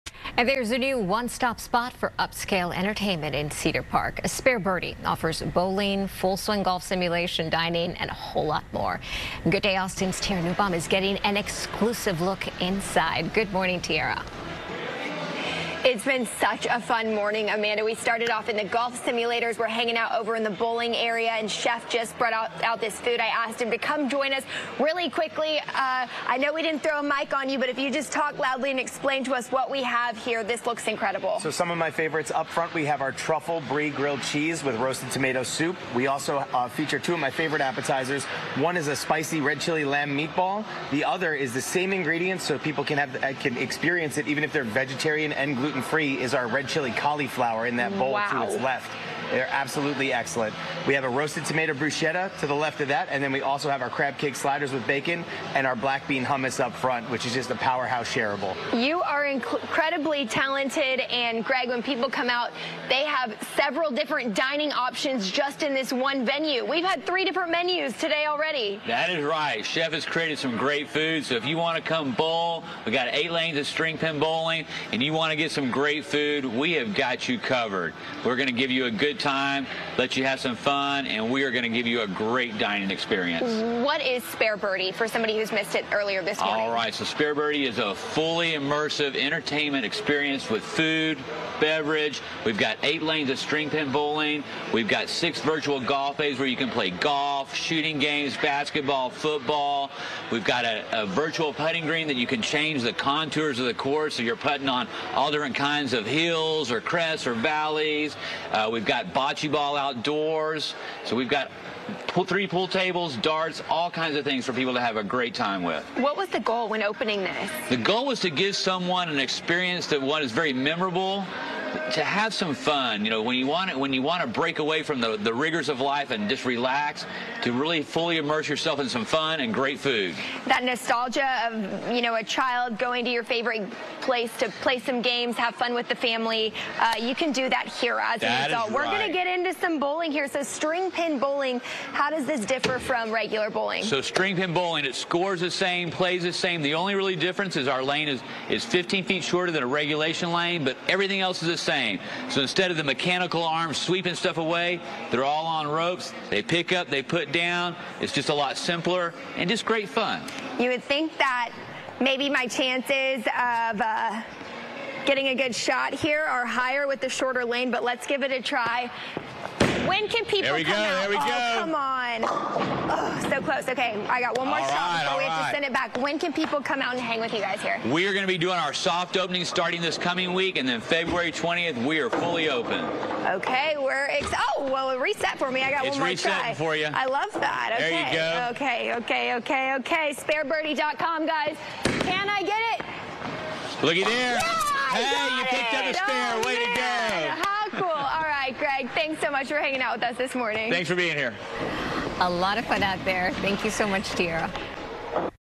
Cedar Park gets new upscale entertainment center | FOX 7 Austin
Spare Birdie features string pin bowling, augmented reality golf, an indoor putting green, and more.